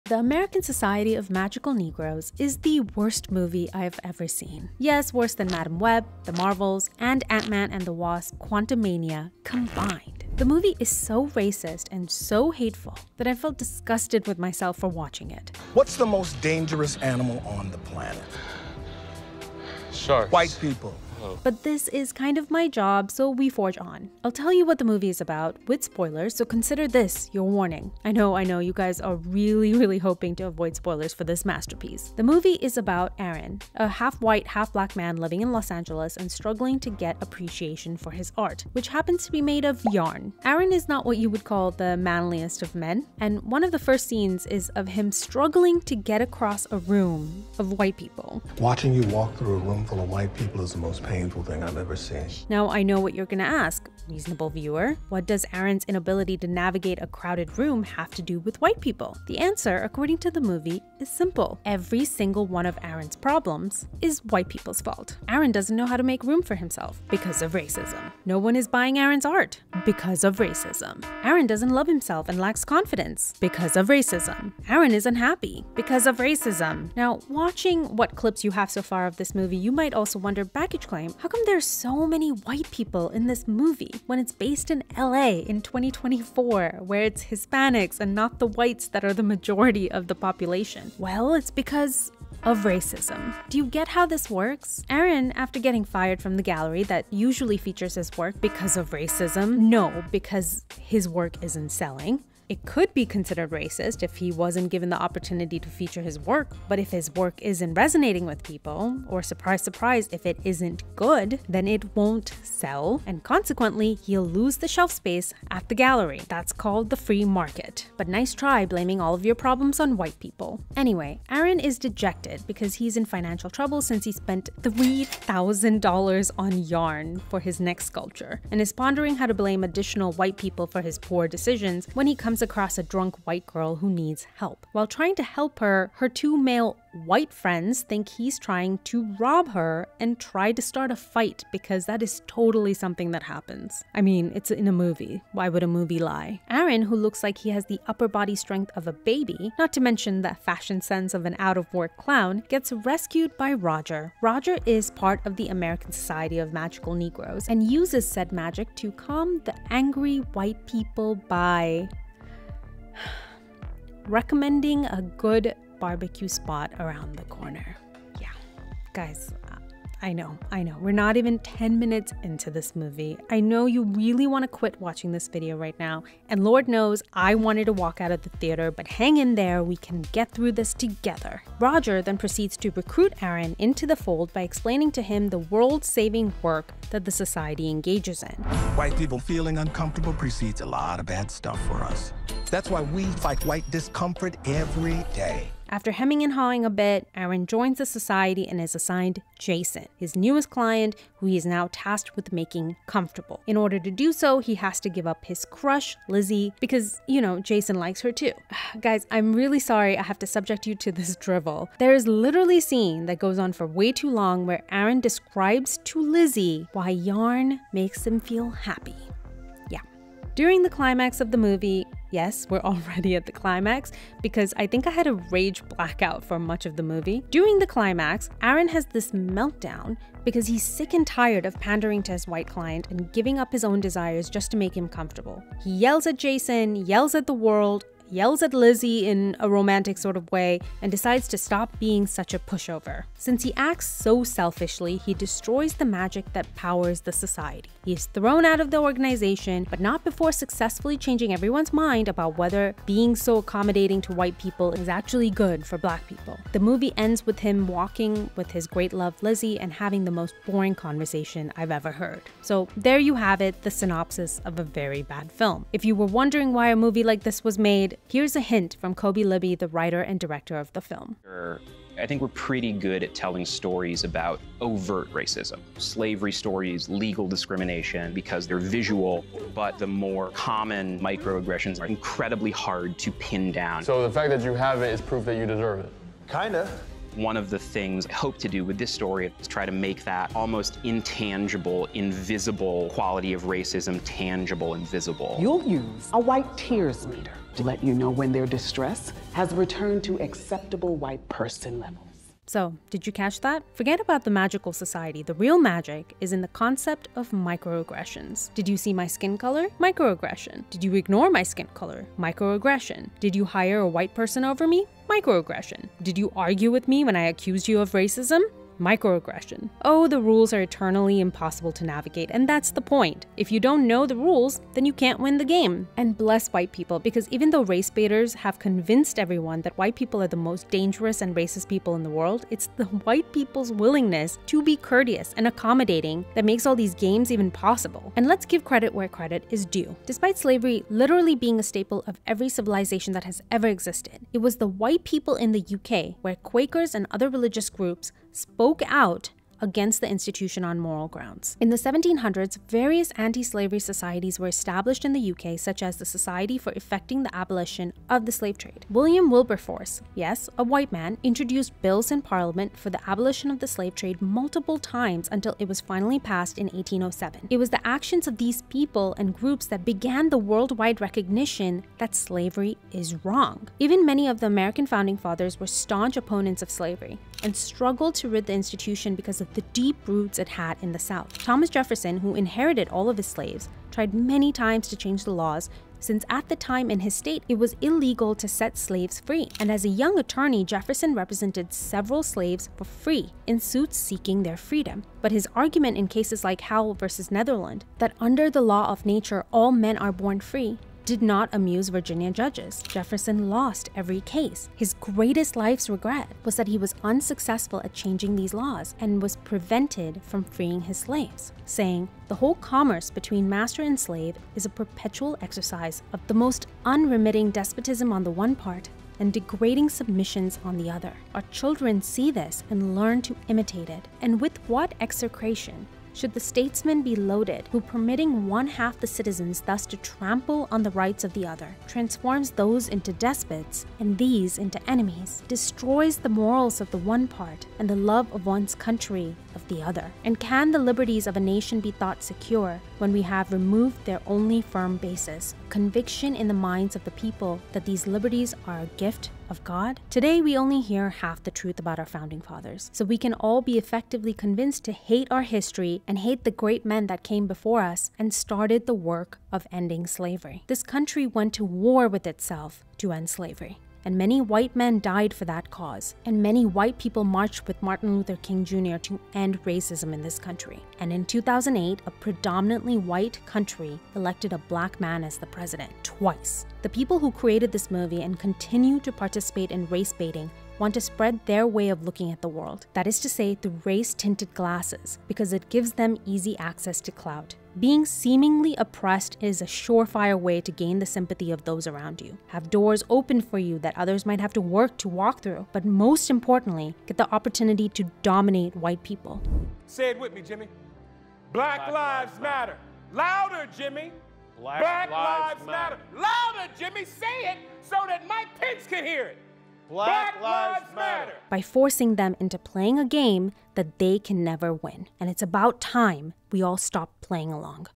Hollywood is obsessed with race-baiting...here's why
The American Society of Magic Negroes is hell bent on spreading intense hatred for White people and America.

In this video, I break down the movie and it's many many lies.